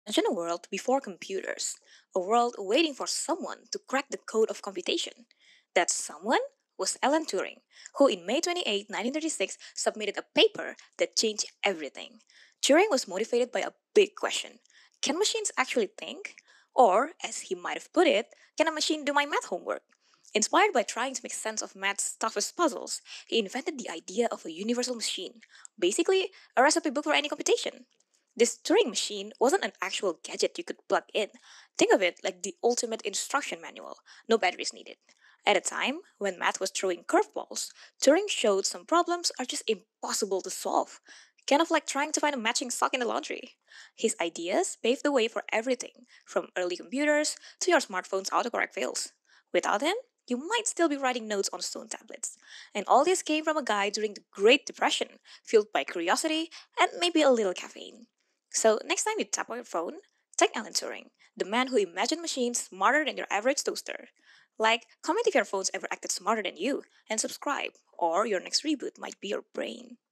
How Alan Turing Imagined the First Thinking Machine
How Alan Turing imagined the first thinking machine! 🤖🧠 Decades ago, he laid the groundwork for computers that could solve complex problems and process information in new ways. Fascinating insight into computing history!